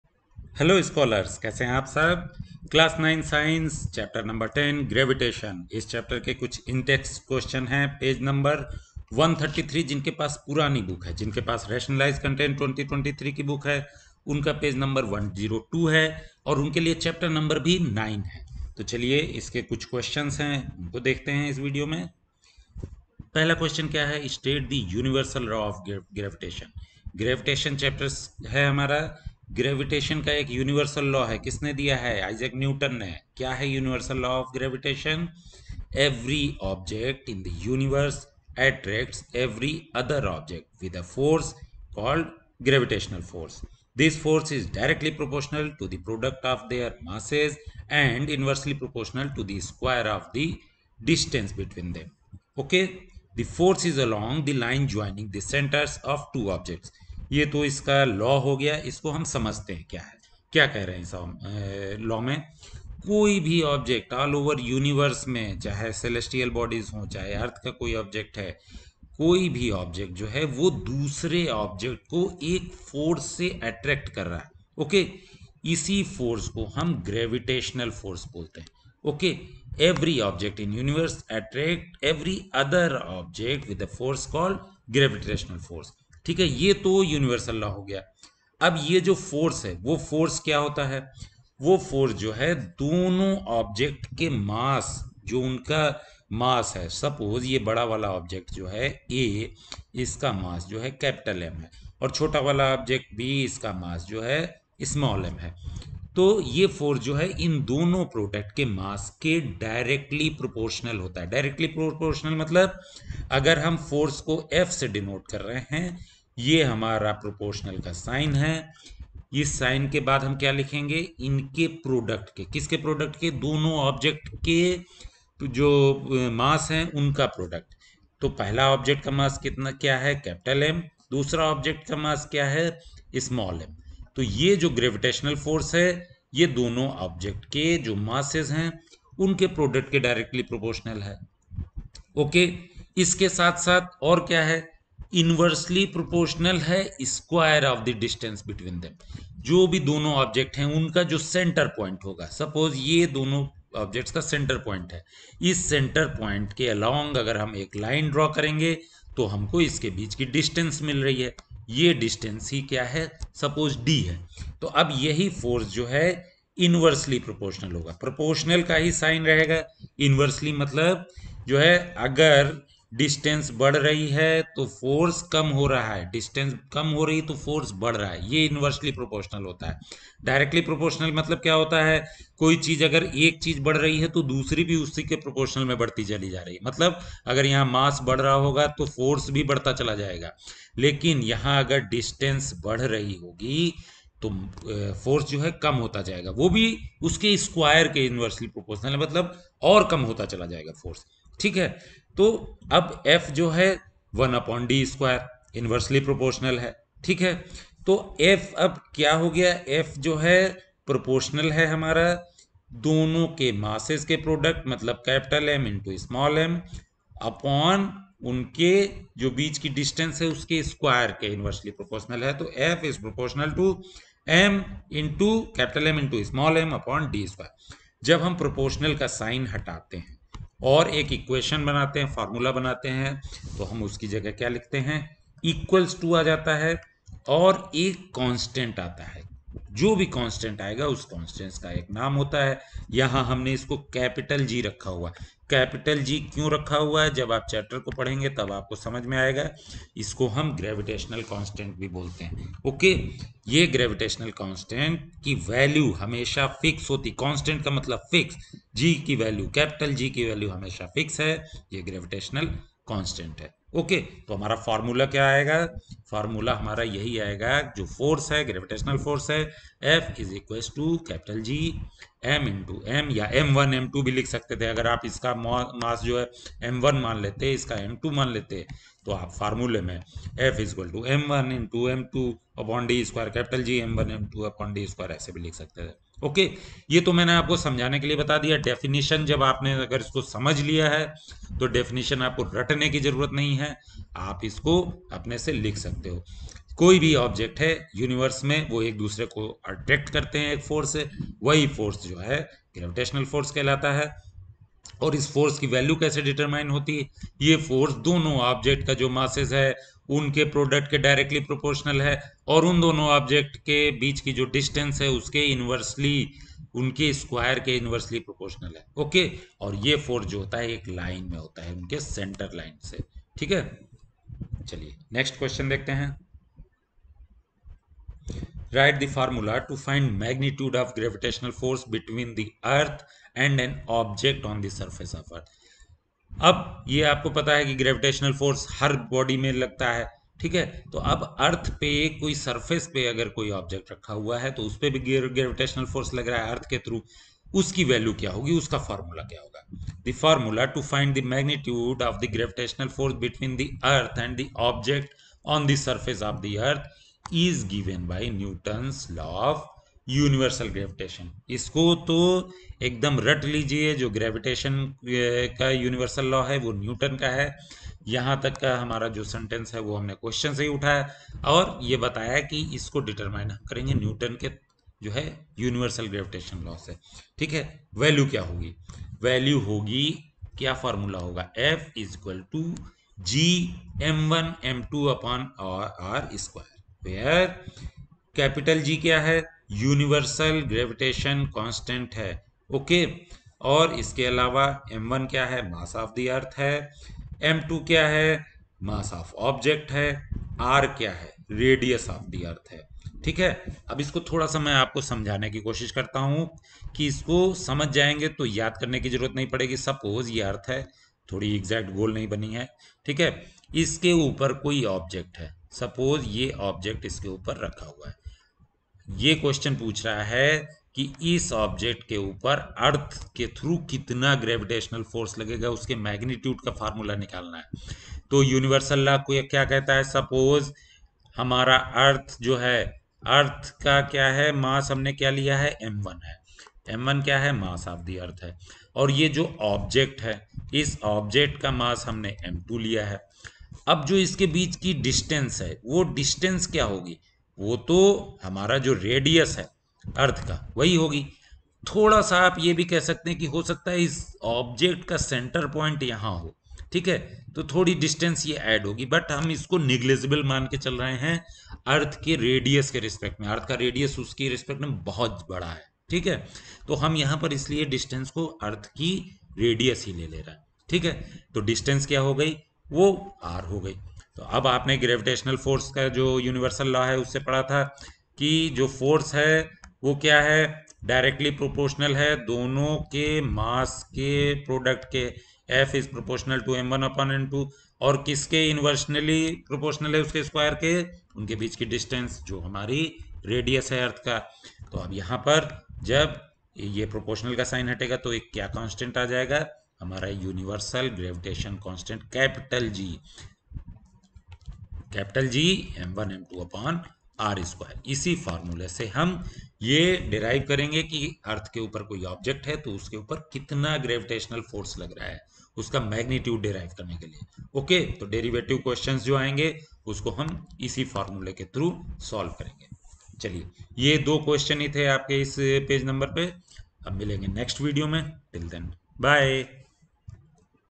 Intext questions Chapter 9 Gravitation Class 9 Science NCERT Page No 102
NCERT solutions for class 9 science chapter 10 Gravitation (Old book)
NCERT solutions for class 9 science chapter 9 Gravitation (New book)
Welcome to our YouTube video on NCERT Class 9 Science Chapter 9 Gravitation, where we will be discussing the answers to the questions given on page no. 134 (old book) / 102 (new book) of this chapter.

In this video, we will cover intext questions of class 9 science chapter 10 page no. 134 (old book). Class 9 science chapter 9 page no. 102 (new book)

I will explain each question in a simple and easy-to-understand manner, using examples and illustrations that will help you grasp the concepts better. This video is especially helpful for those who are preparing for their Class 9 Science exams and want to score good marks.
So, whether you are a student or a teacher, this video is a must-watch for anyone who wants to understand the concept of gravitation in a better way. So, sit back, relax, and enjoy our NCERT Class 9 Science Chapter 9 gravitation video.
1. State the universal law of gravitation.
2. Write the formula to find the magnitude of the gravitational force between the earth and an object on the surface of the earth.